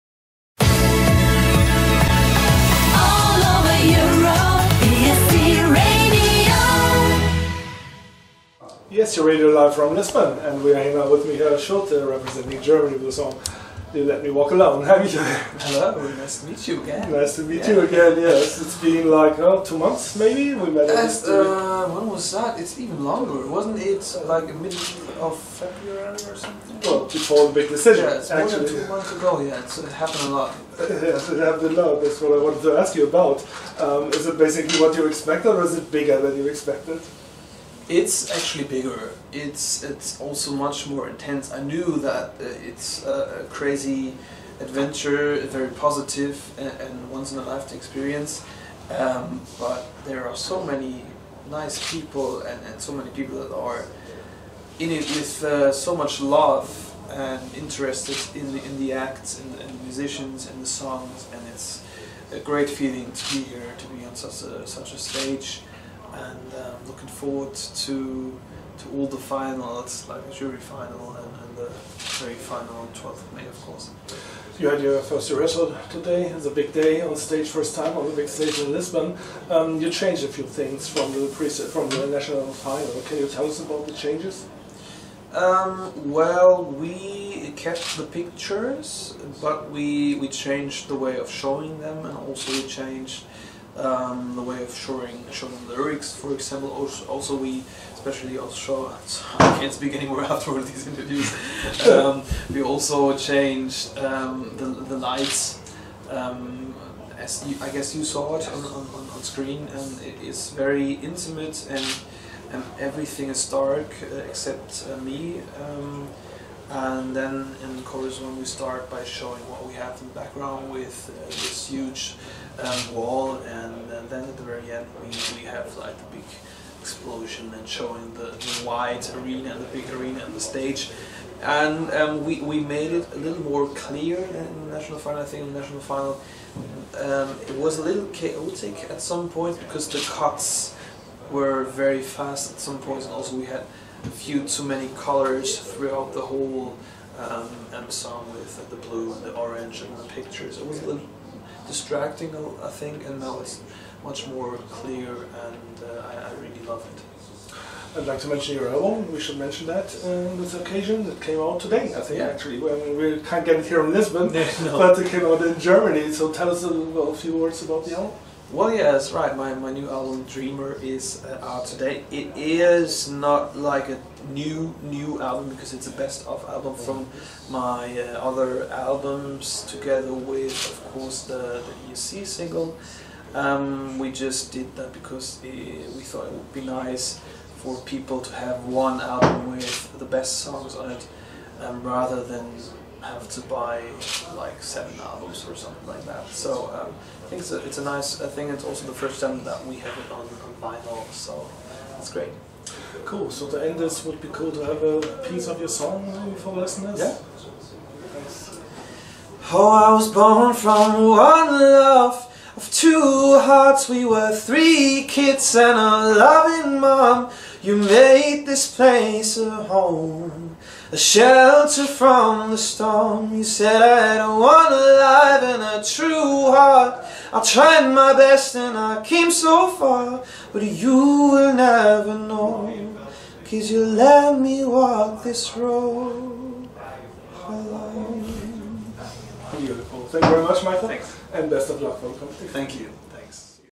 Michael Schulte (Germany) interview @ Eurovision 2018 Lisbon | ESC Radio
Interview with Michael Schulte (Germany) @ Eurovision Lisbon 2018